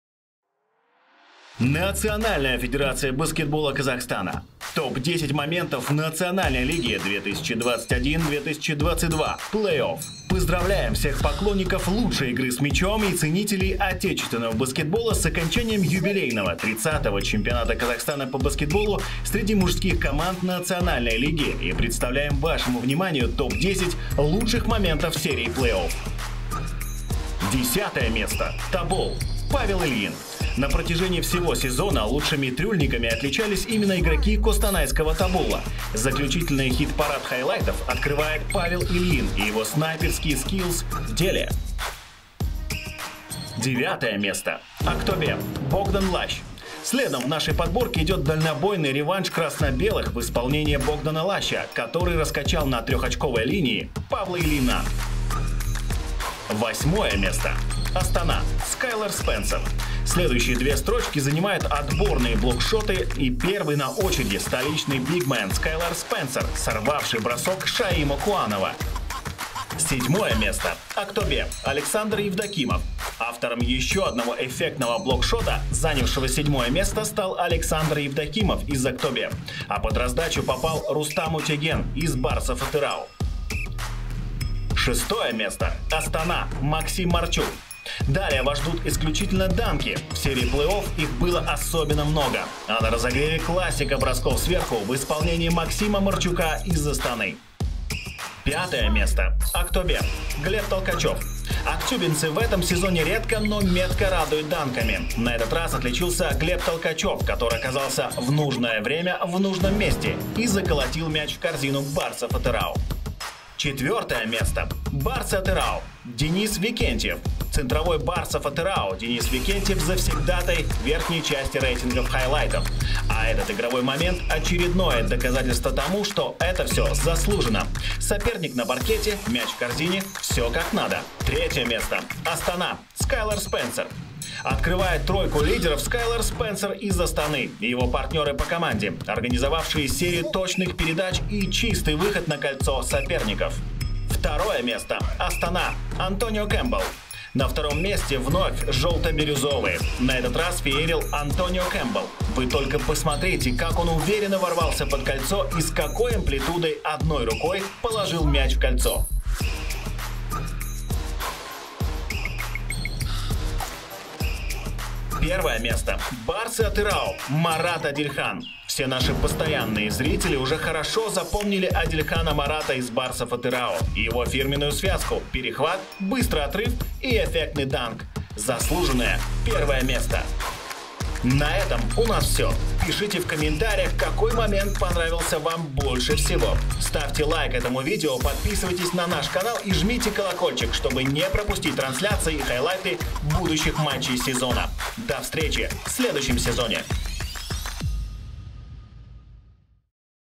🏀 TOP-10 моментов серии плей-офф Национальной лиги 2021/22
👏🏻 Еще раз поздравляем всех любителей казахстанского баскетбола с окончанием юбилейного XXX Чемпионата среди мужских команд Национальной лиги!
🔝 Представляем вашему вниманию ТОП-10 лучших моментов серии плей-офф от телеканала INSPORT и букмекерской компании 1XBET.
💬 Пишите в комментариях, какой момент понравился вам больше всего?